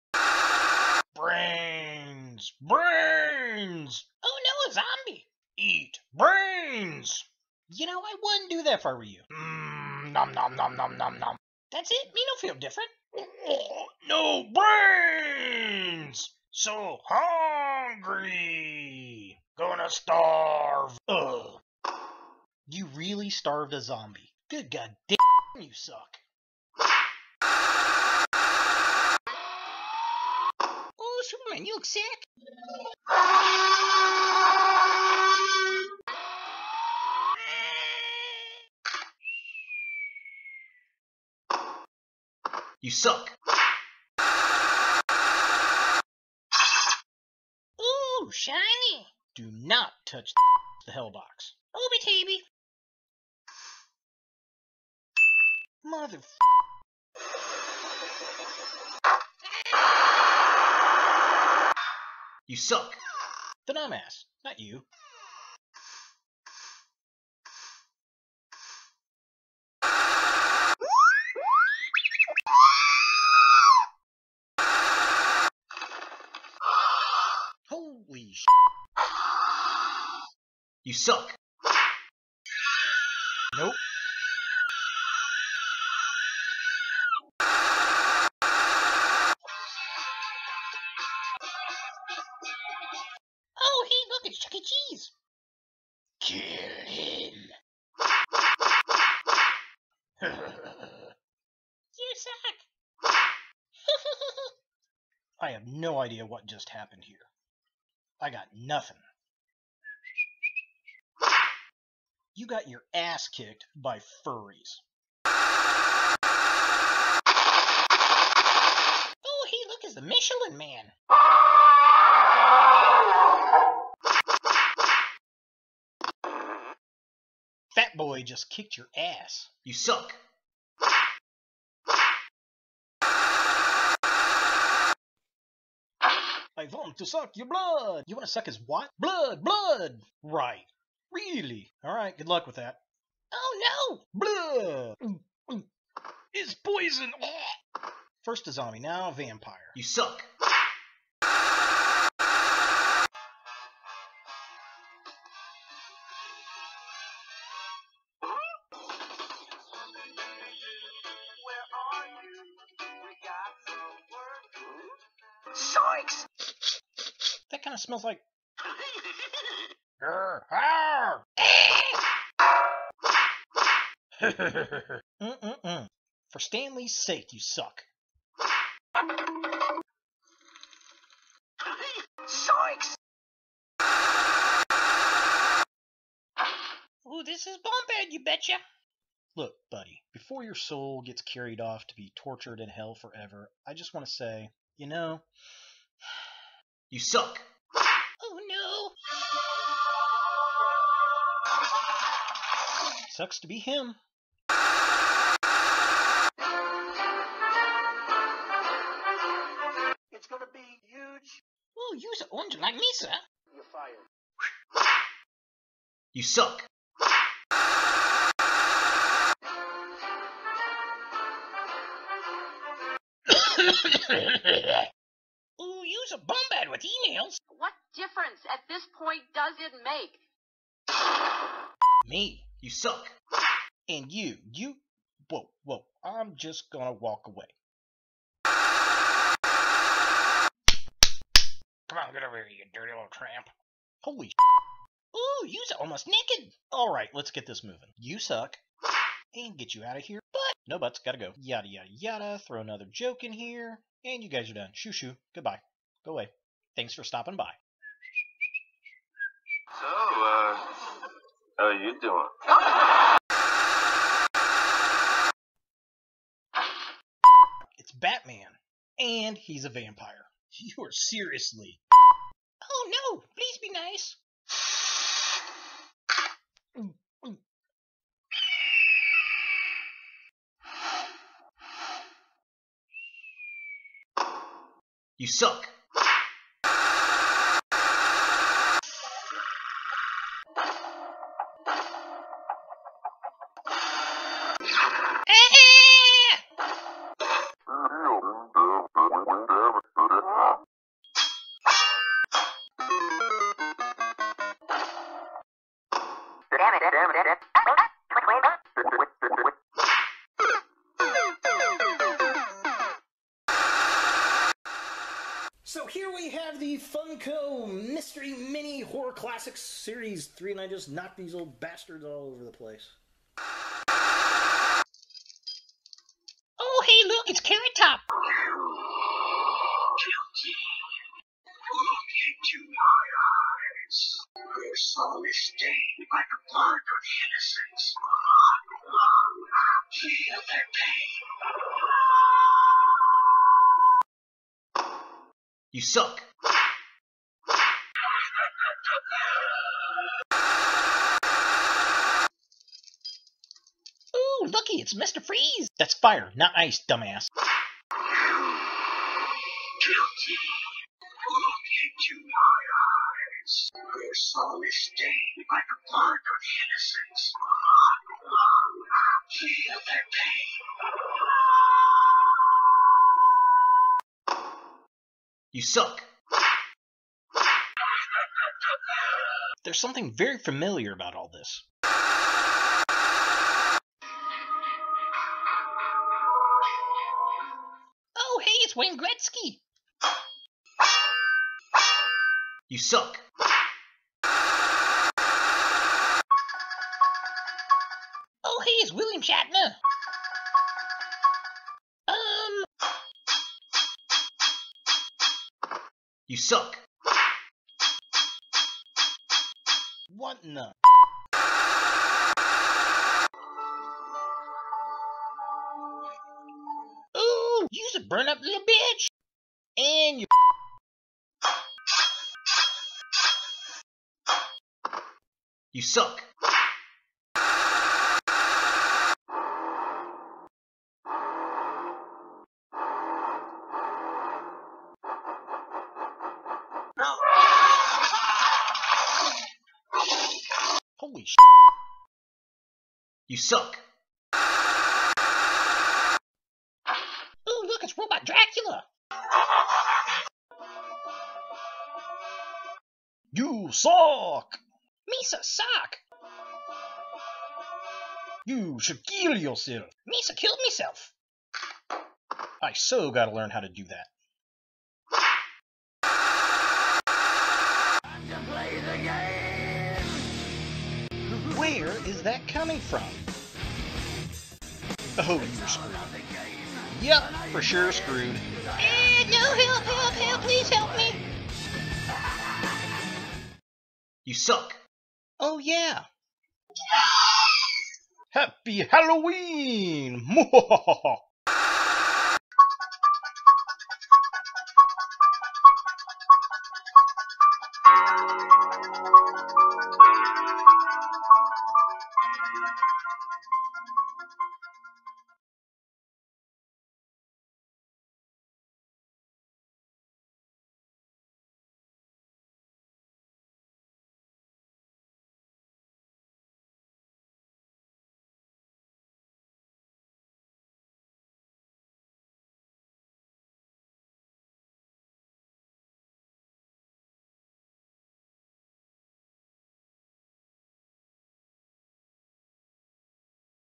Gag Reel Volume 13 Halloween Special 31 Horror Toys in 31 Days
This is a collection of stop motion animated shorts from the month of October (2016) for Halloween & the 31 Horror Toys in 31 Days!